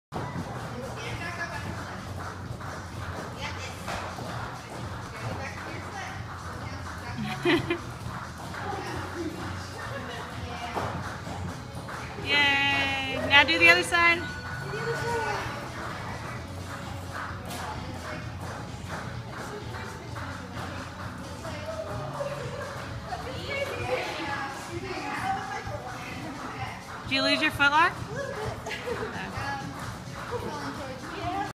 Aerial Silks - rotisserie chicken
That's what my teacher told me this move was called. I dunno its fun tho!